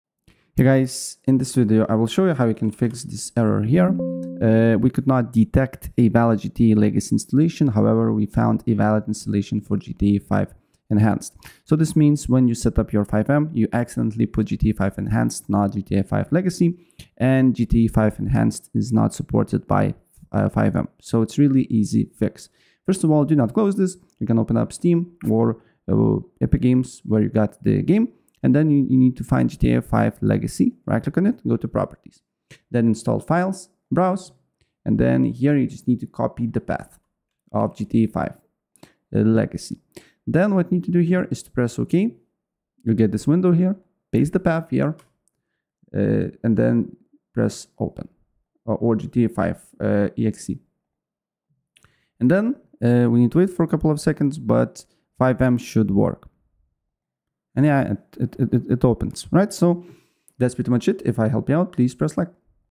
Fix We Could Not Detect a Valid GTA V Legacy Installation on FiveM
In this video I will show you how to Fix We Could Not Detect a Valid GTA V Legacy Installation on FiveM. It's really easy and it will take you less than a minute to do it!